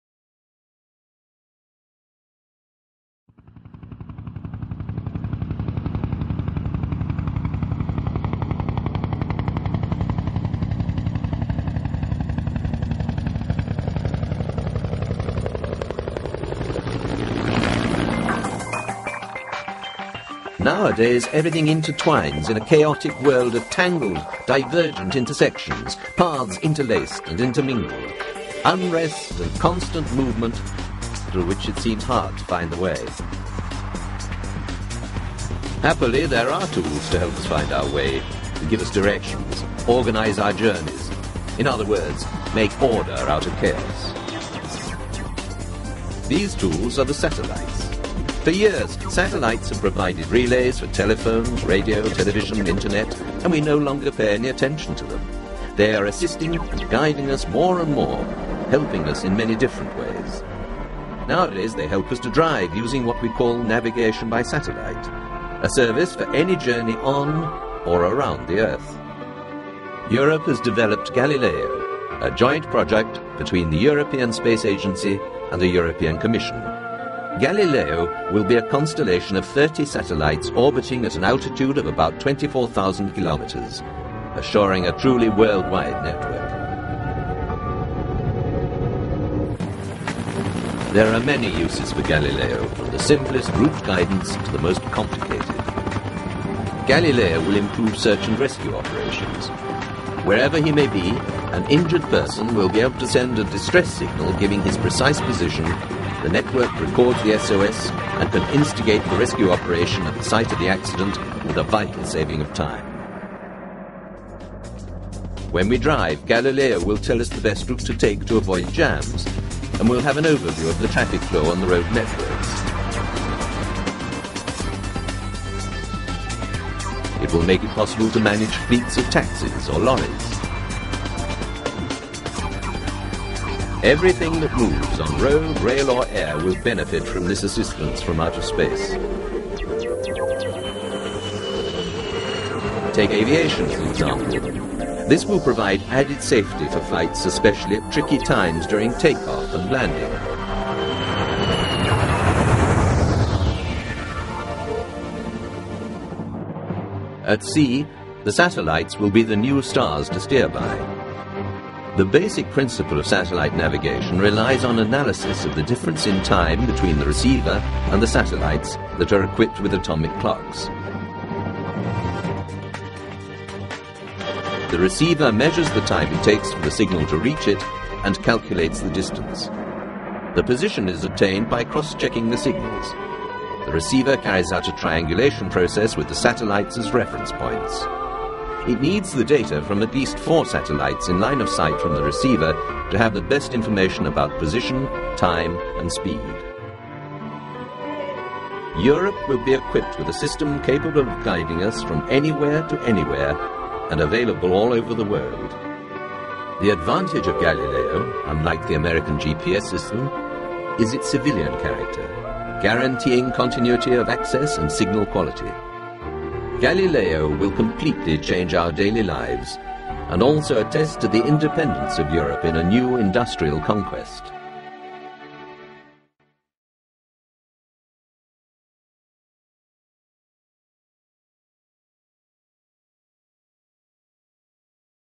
Galileo -- Europe leads the way... in 3D!
A 3D presentation of the Galileo satellite navigation system, a joint development by the European Union and European Space Agency. This constellation of satellites will allow users to pinpoint their location anywhere on Earth -- their launches commencing on 20 October 2011.

Use red/blue 3D glasses to experience the full 3D experience of this clip.